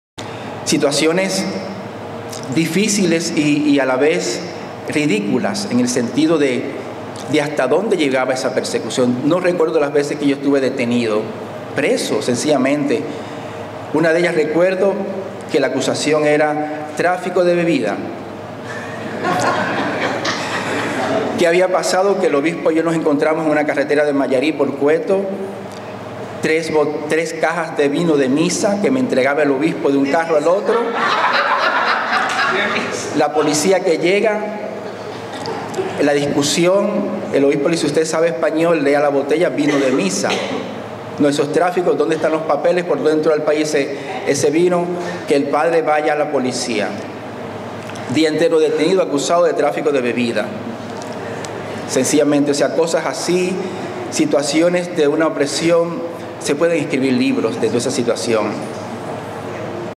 Vicario de la Ermita de la Caridad de Miami fue detenido en Cuba por tráfico de bebidas
El Padre Céspedes, Vicario de la Ermita de la Caridad de Miami, relata como fue detenido en Cuba y acusado de tráfico de bebidas. Para ver su intervención completa visite